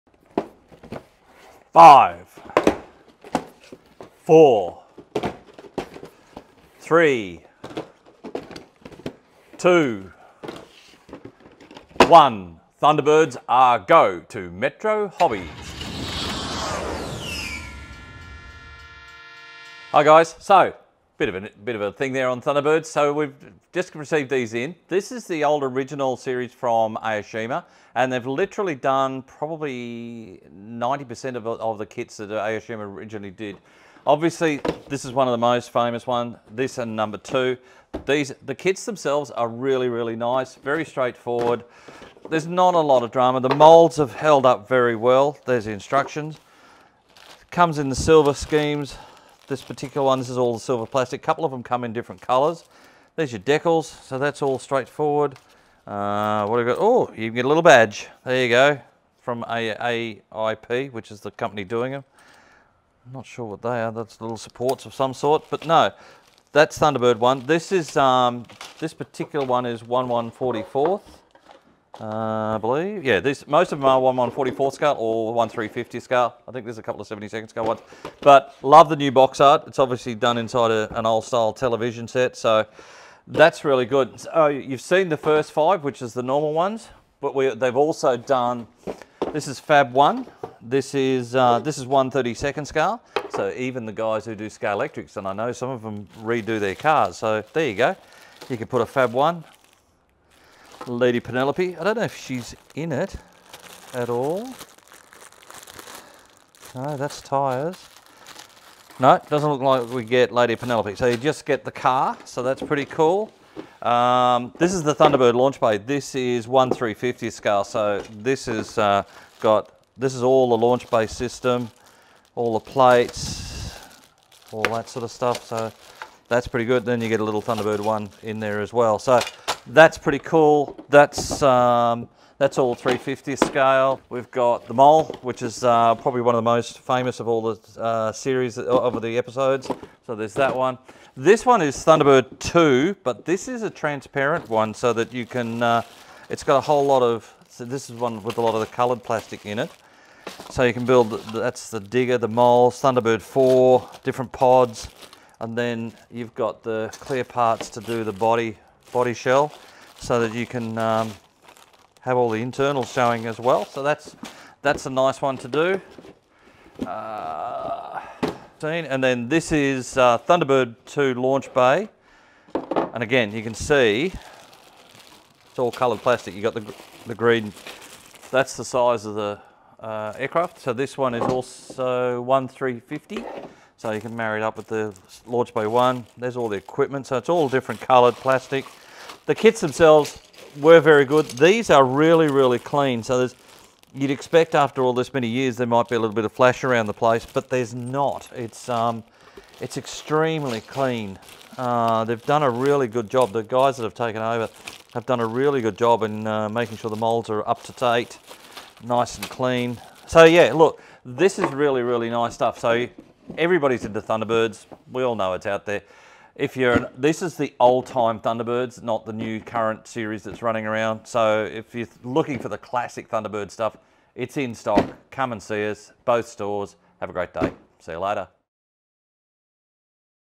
The Thunderbirds are go! go! go! And here at Metro Hobbies!
Gerry Anderson's iconic 1960s science fiction TV adventure series produced such legendary aircraft and vehicles such as The Mole and Fab One, as well as all five of the Thunderbirds. These are some of the most recognizable sci fi designs to hit the small screen, and everyone fondly remembers their favourite. Well, Max launches us into a full spread of all the AIP Thunderbirds series re-issues in the shop! Tune in and launch into the wild blue because these kits are all going to go! go! go!